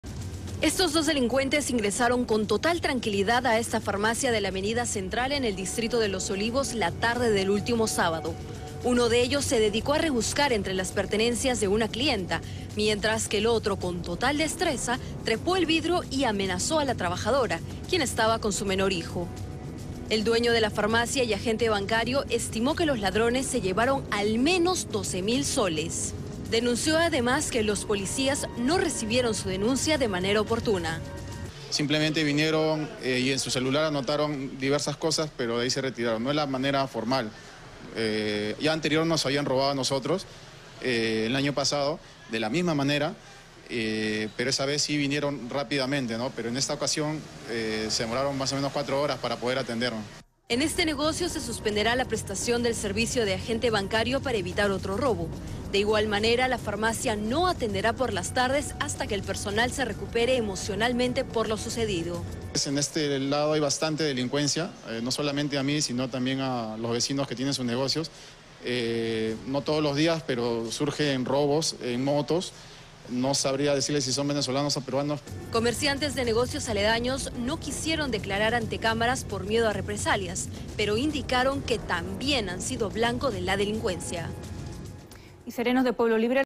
Asaltan botica y se llevan 12 mil soles en Los Olivos
En el distrito de Los Olivos una banda de delincuentes ingresó a una botica donde también opera un agente bancario.

Uno de los delincuentes saltó el mostrador del negocio, y con el arma de fuego amenazó a los trabajadores, mientras su cómplice lo hacia con los dos clientes que estaban en la botica.

Tras lograr amedrentarlos, los ladrones se apoderaron de las ganancias del día, y del dinero de la caja del agente bancario que funciona en el lugar, en total se llevaron más de 12 mil soles en efectivo.

El propietario de la botica pide mayor presencia policial en la zona, que se ha vuelto el blanco de la delincuencia en esta parte de la capital.